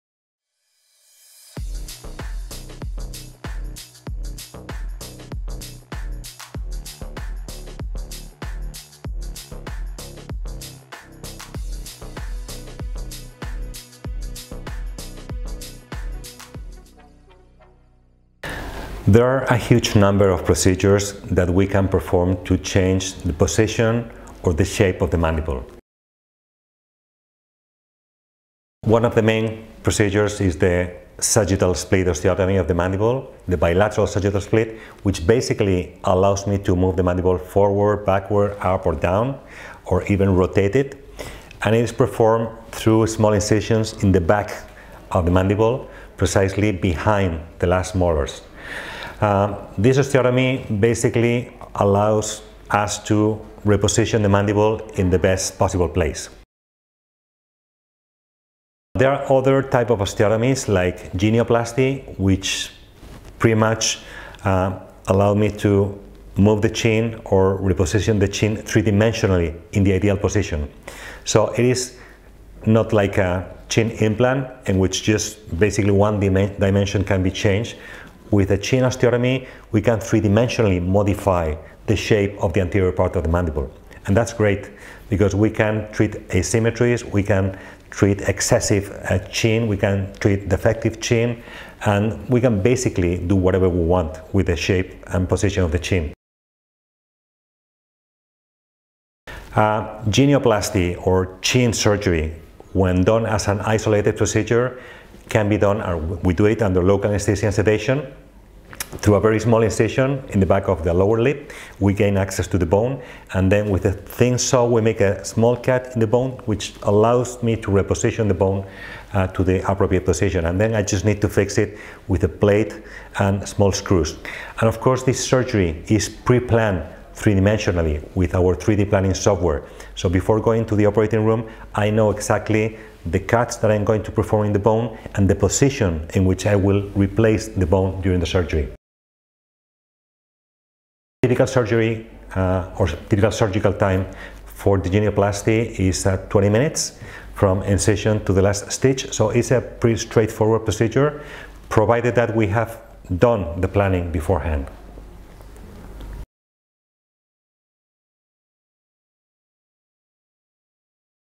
Jaw Reshaping
Prof. Federico Hernández Alfaro explains different techniques to change the shape of the jaw, their advantages and differences.